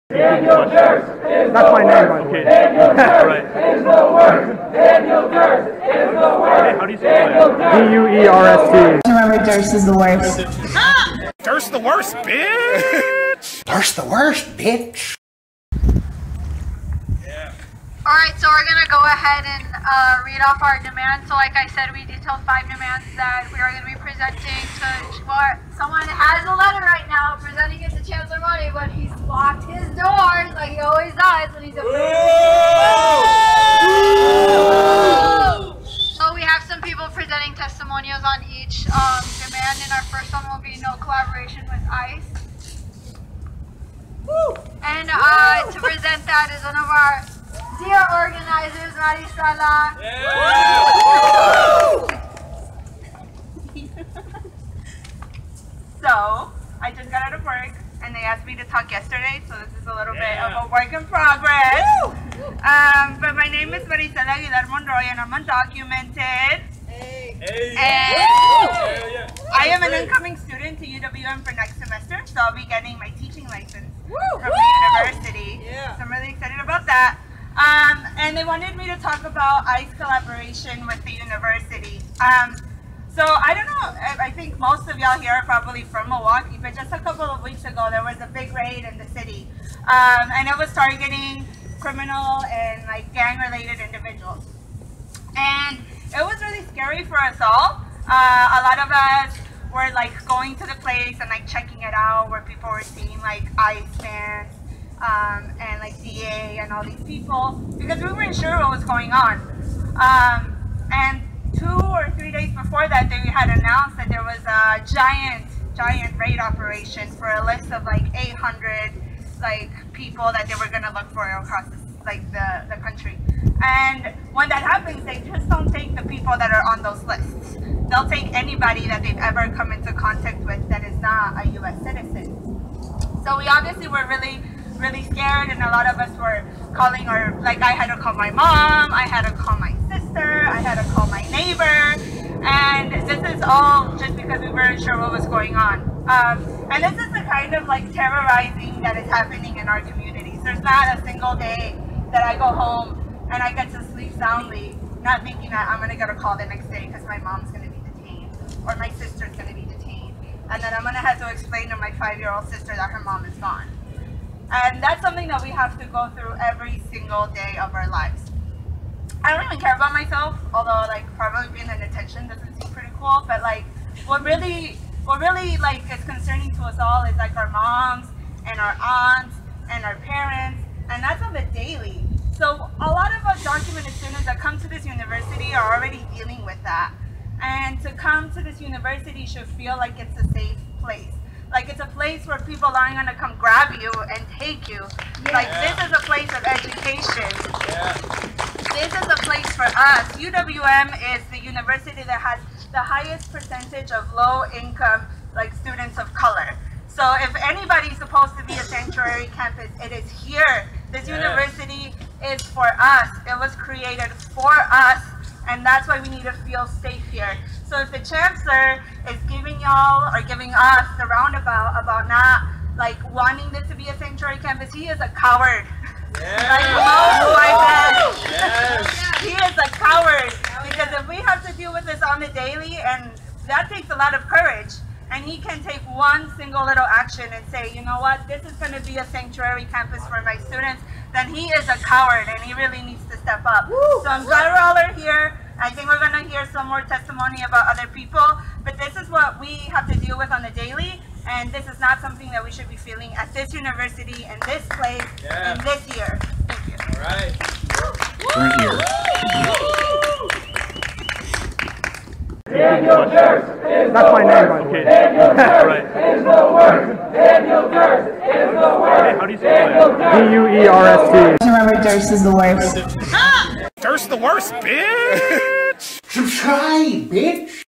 "Rally to Announce UWM Sanctuary Campus Demands" Speaker #1
The 1st in a series of speeches from the "Rally to Announce UWM Sanctuary Campus Demands."   Enjoy.

BITCOIN Wallet:  1MqqgrahFutJi5DVgR1eQ1c9voN9m1pBhk

Download Robin Hood stock trading app and get free stock just for signing up